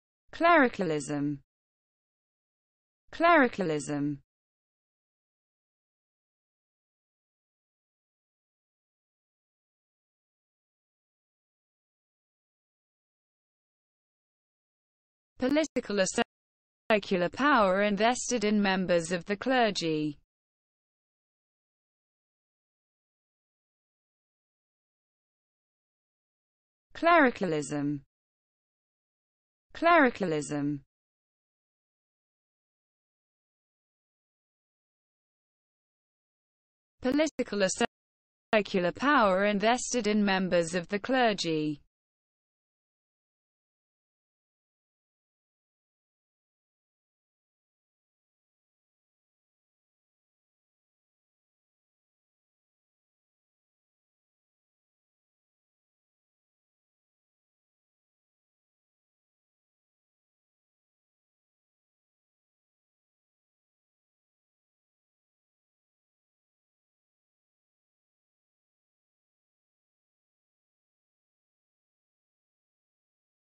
What does Clericalism mean?
What does Clericalism mean in English? Learn the meaning of Clericalism.